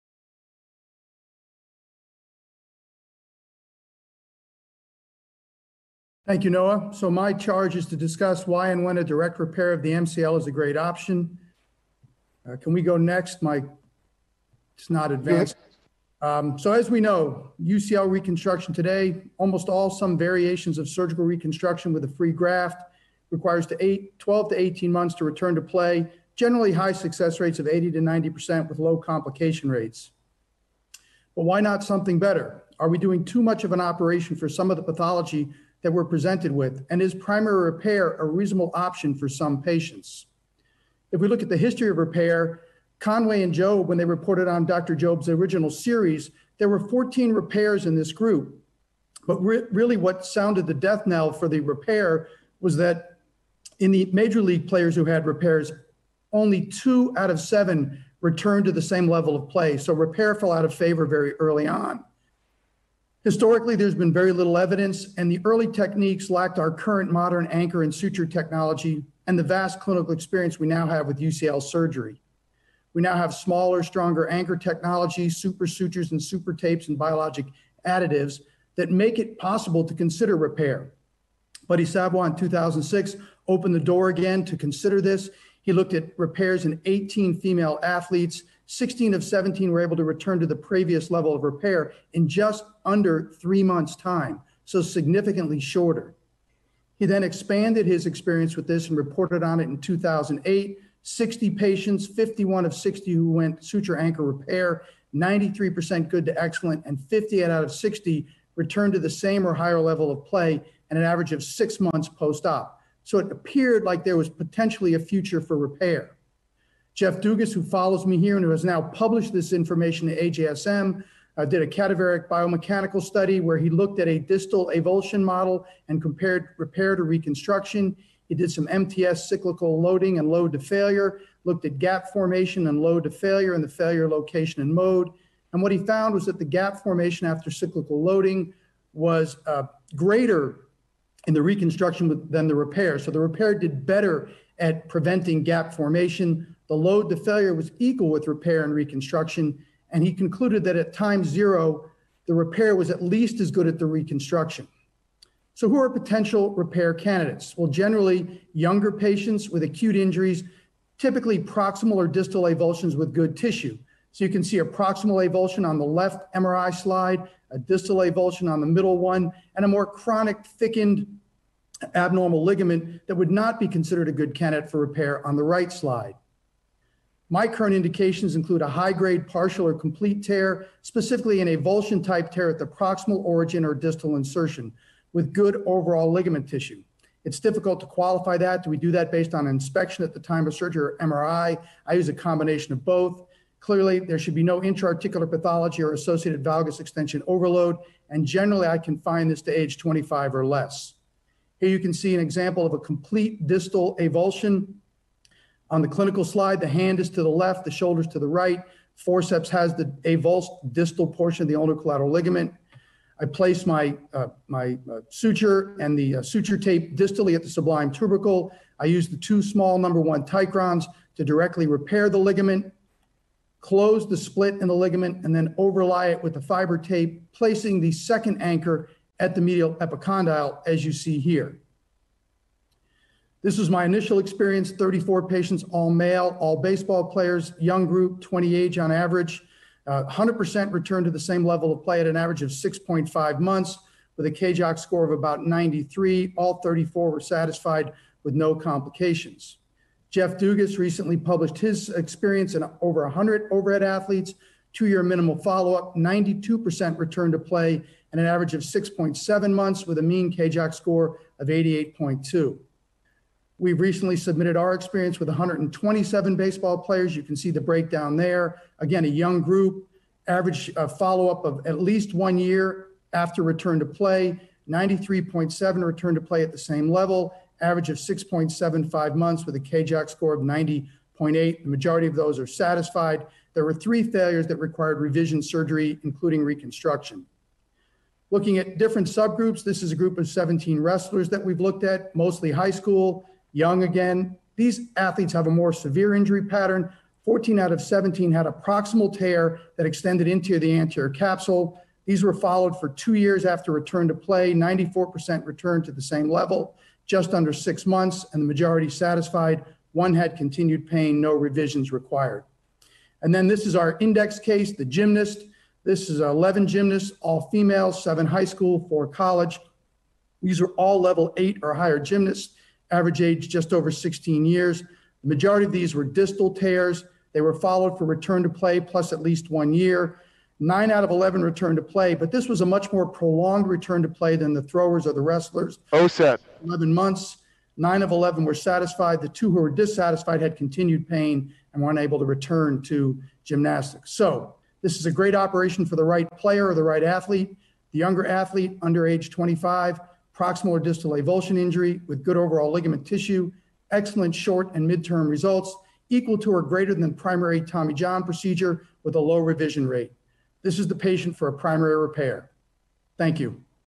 Pro: Why & When A Direct Repair Of The MCL Is A Great Option - George A. Paletta, MD
This video is from the 2020 Orthopaedic Summit Evolving Techniques.

If you'd like to learn more about Medial Ulnar Collateral Ligament Injury, click here to view the topic on Orthobullets